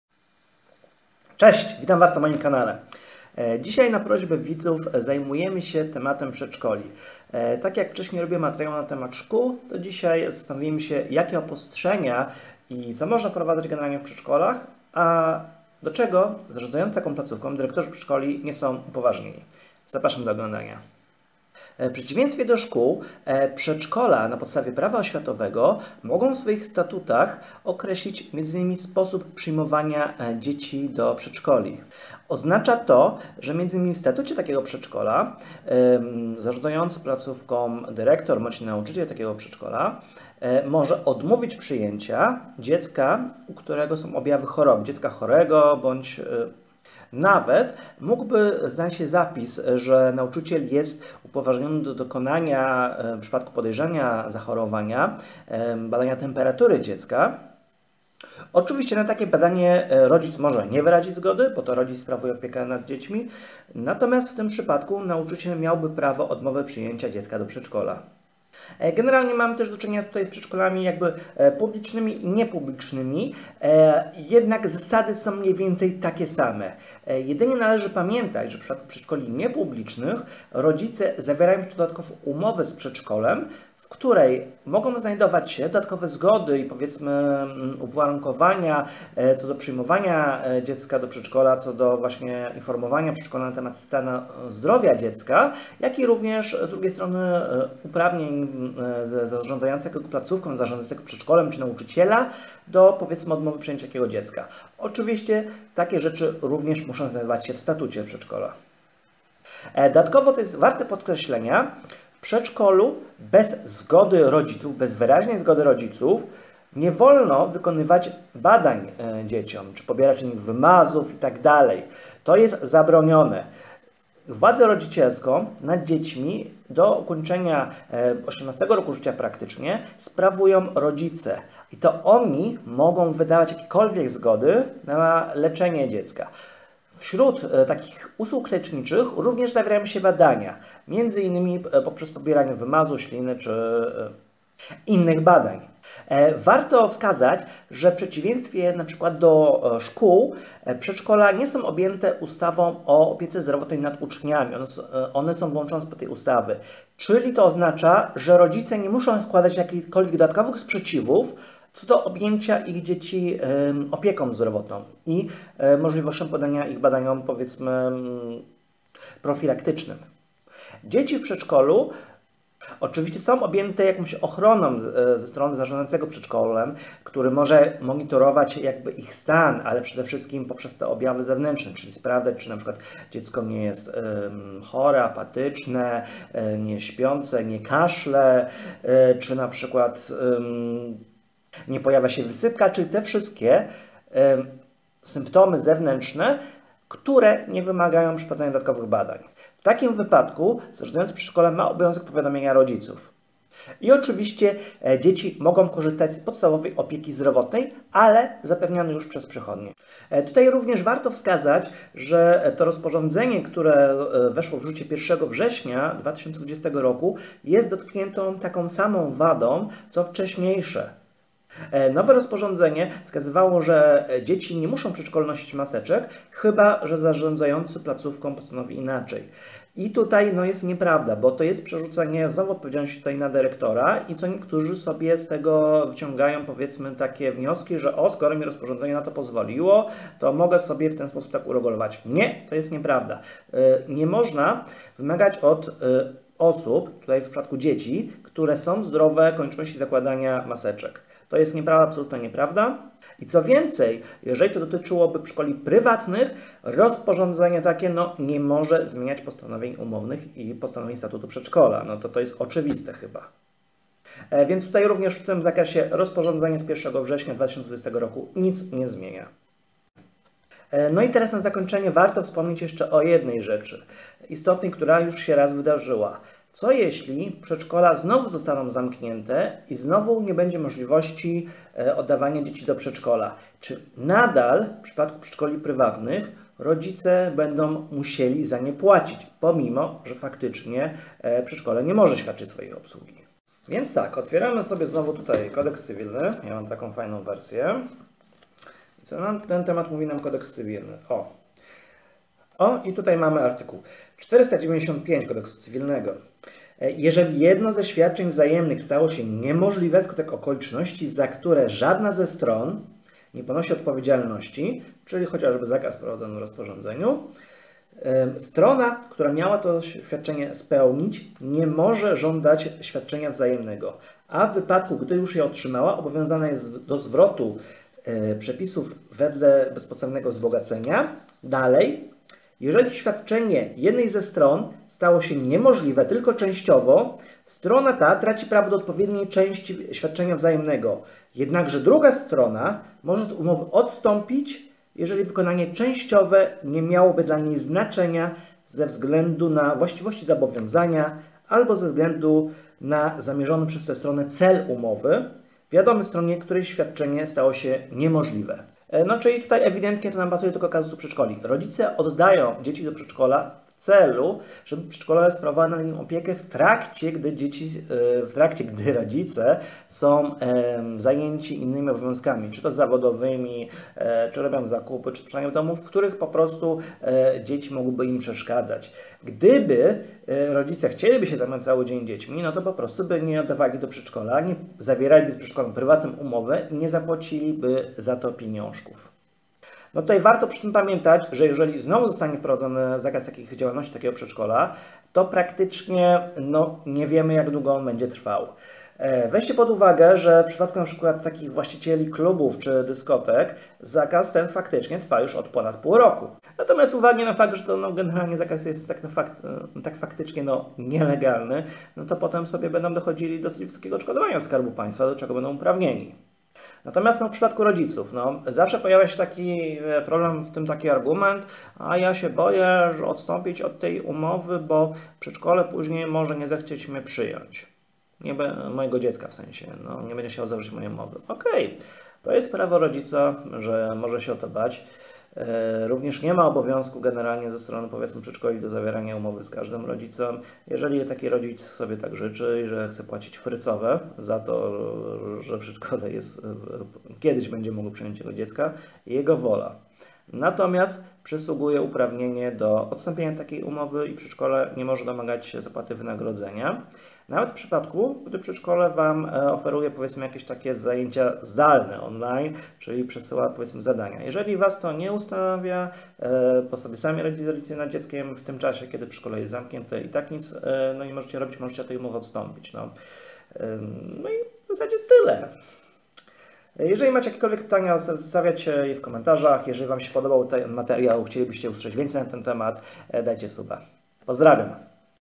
Czy obostrzenia w przedszkolach są legalne? Czy trzeba płacić za przedszkole jak jest ono nieczynne?
Prawnik opowiada czy obostrzenia wprowadzane w przedszkolach są legalne. Co mogą robić dyrektorzy i nauczyciele, a czego nie wolno? Czy w przypadku zamknięcia przedszkola nadal trzeba za nie płacić?